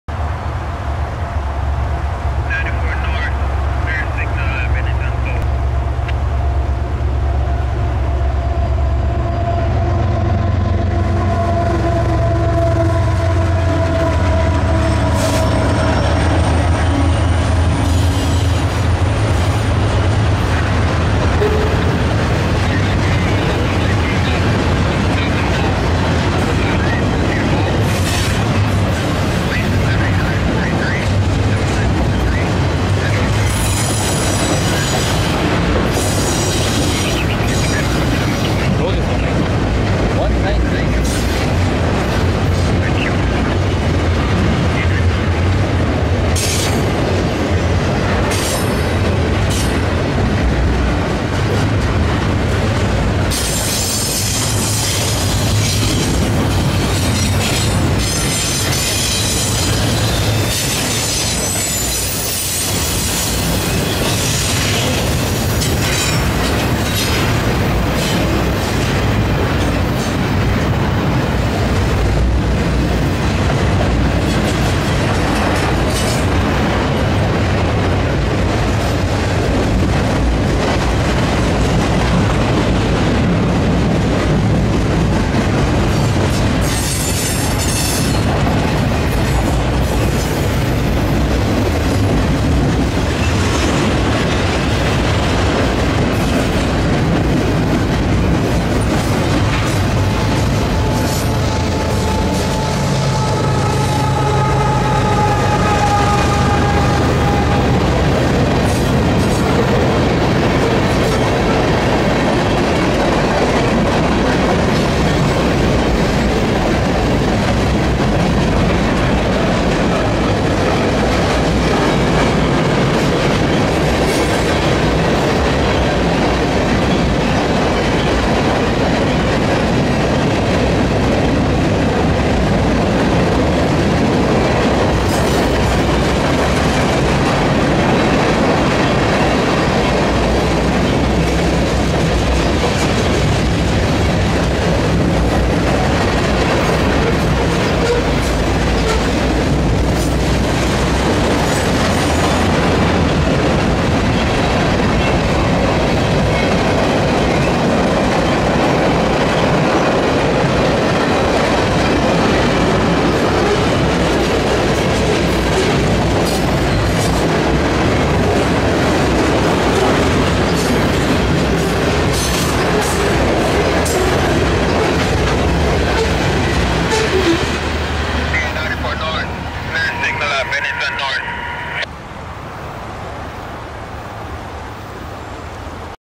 CP Rail Northbound Mixed Freight
Date: 6/18/22
Location: Confluence Park Calgary, AB
MP: N/A
Subdivision: Red Deer Sub
Train ID: 119
Locomotives:
CP 8894 ES44AC
CP 7046 SD70ACU
CP 8789 ES44AC Mid-DPU
Side Notes: